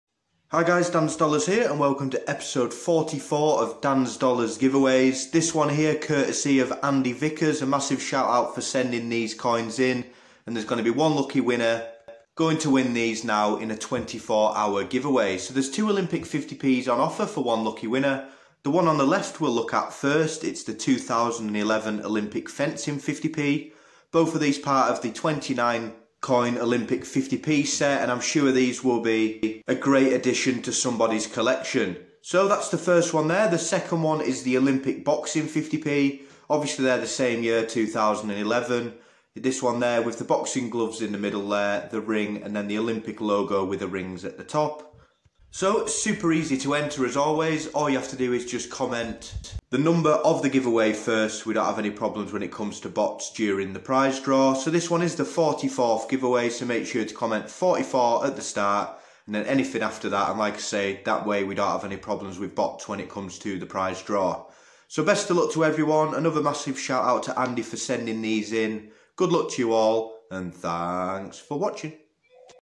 YOUTUBE GIVEAWAY OPEN #44 - 2 OLYMPIC 50P COINS From ANDIE VICKERS!!!
THIS GIVEAWAY SPONSORED BY ANDIE VICKERS features the 2011 boxing 50p coin and the 2011 fencing 50p coin Its so easy to enter just start your comment with #44 followed by anything else. Please start it with #44 to keep the bots away.
YOU HAVE UNTIL 6PM 20TH AUGUST 2020 TO ENTER
THANKS FOR ALL THE SUPPORT EVERYONE!